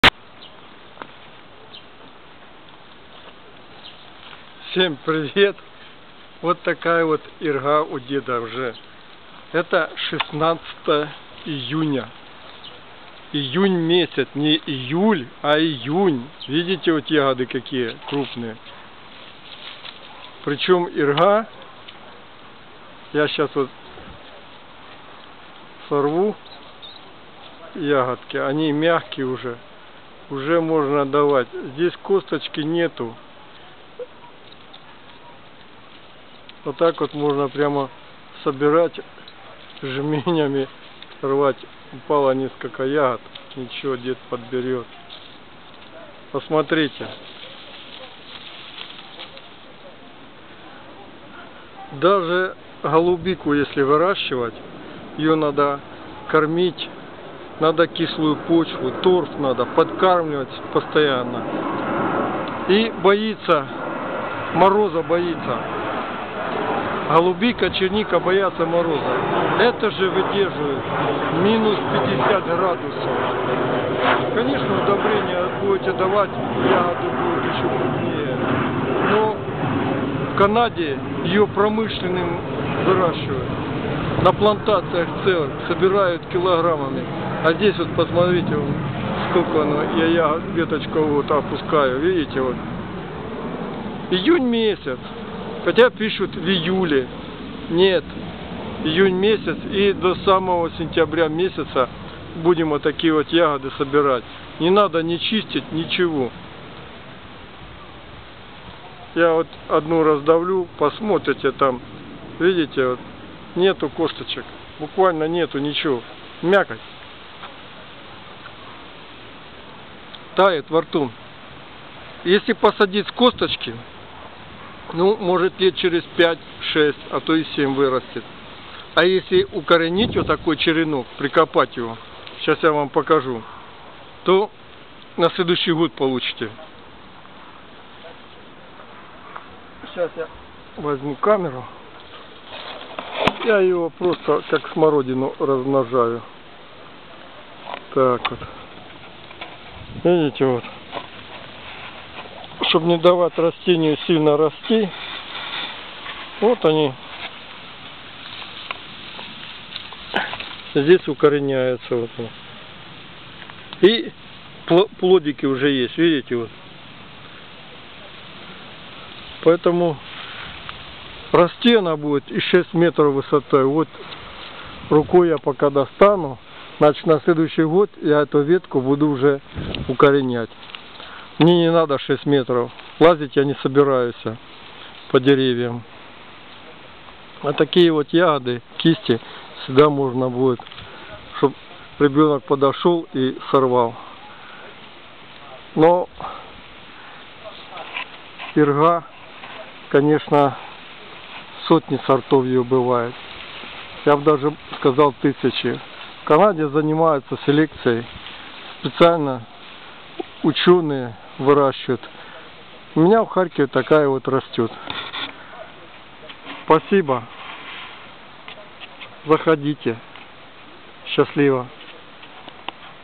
ИРГА в июне !!!! Канада, ,Прибалтика , Кавказ,Крым ...... Харьков
Варенье из ягод ирги,компоты,мармелад, .вареники,пирожки,мороженное,напитки .свежая ягода для левитации.Главное,не надо прогибаться, как перед земляникой.